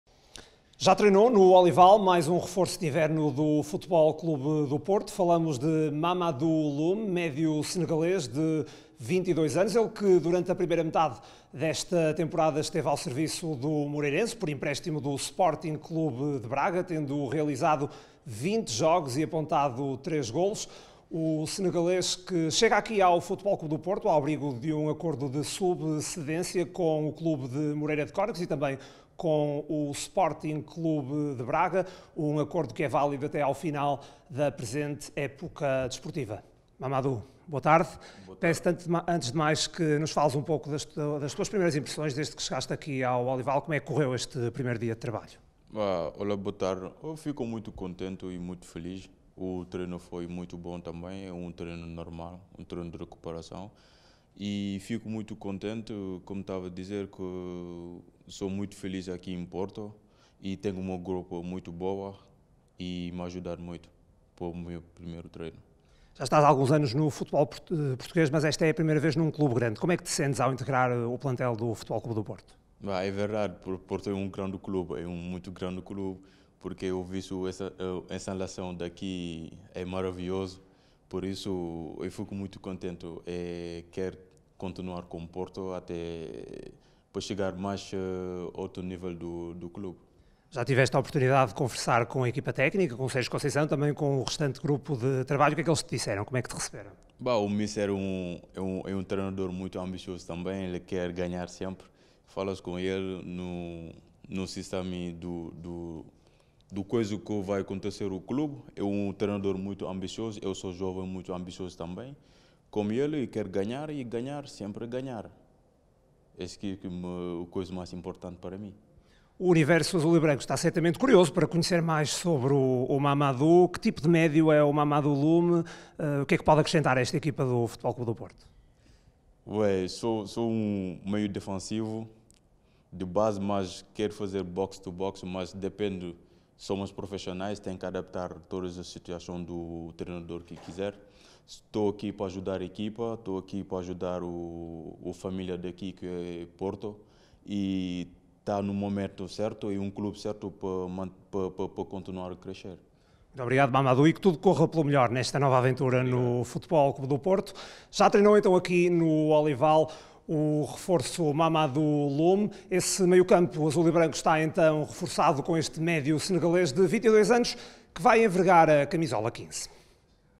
Mamadou Loum: "O FC Porto é um clube muito grande"
Mamadou Loum realizou esta sexta-feira o primeiro treino como jogador do FC Porto. Após a sessão de trabalho no Centro de Treinos e Formação Desportiva PortoGaia, no Olival, o médio defensivo falou ao Porto Canal sobre o novo capítulo na carreira. O jogador senegalês de 22 anos quer continuar a evoluir, agora com a camisola azul e branca, e garante ter sido bem recebido no balneário do FC Porto.